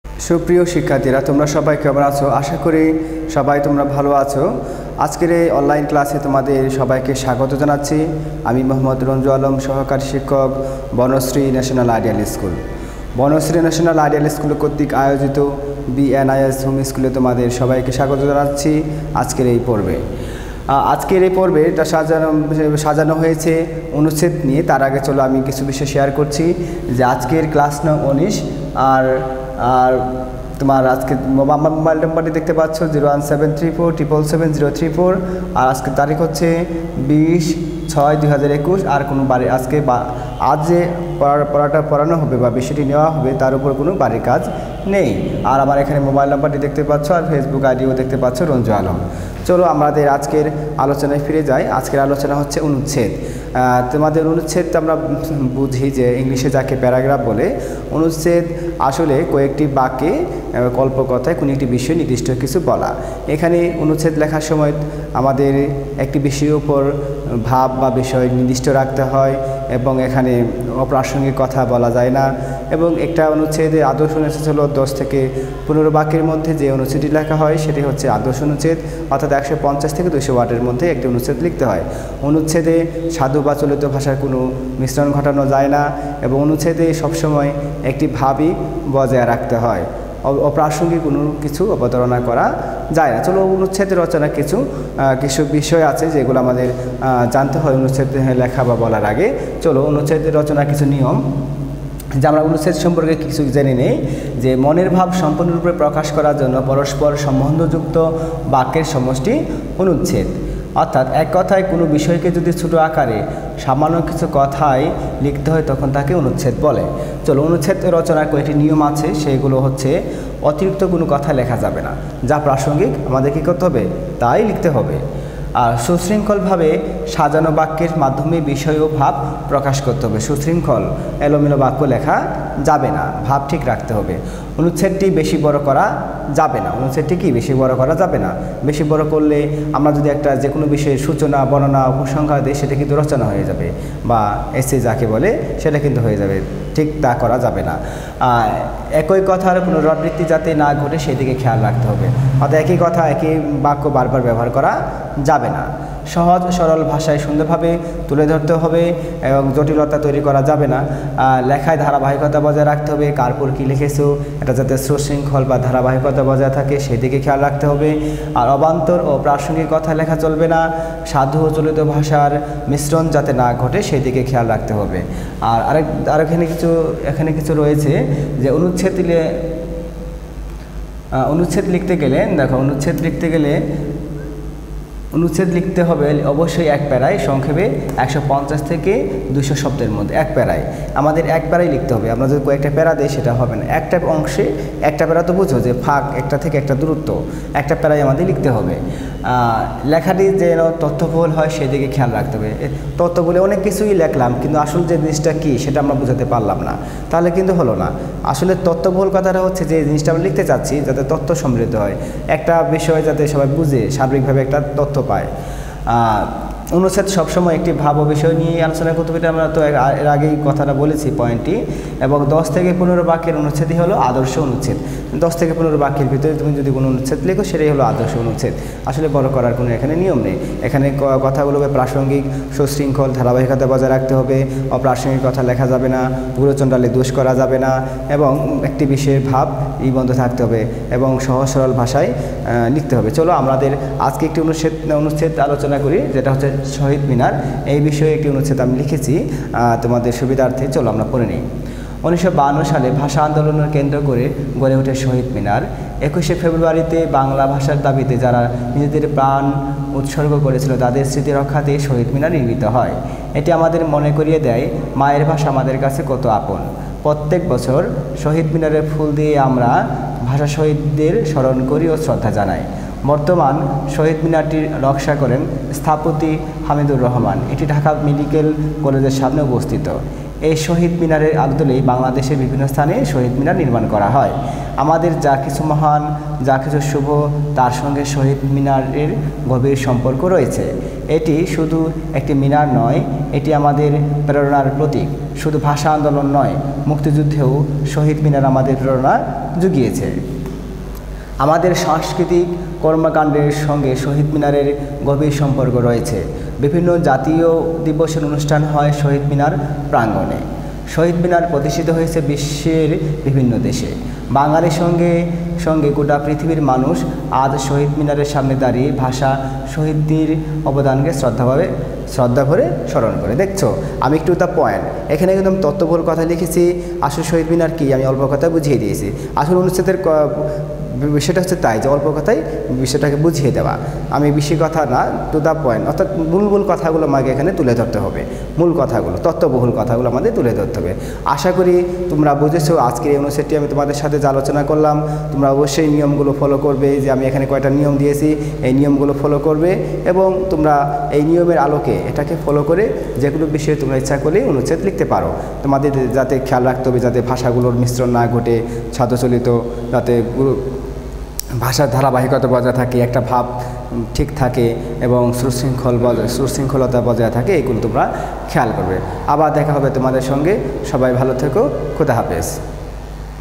সপ্তম শ্রেণির বাংলা ২য় পত্র, ক্লাস নং-১৯, আলোচ্য বিষয়: অনুচ্ছেদ শহীদ মিনার
অর্ধবার্ষিক-২০২১
শ্রেণি: সপ্তম
বিষয়: বাংলা ২য় পত্র
ক্লাস নং-১৯
আলোচ্য বিষয়: অনুচ্ছেদ শহীদ মিনার
শিক্ষক: জ. মো: রনজু আলম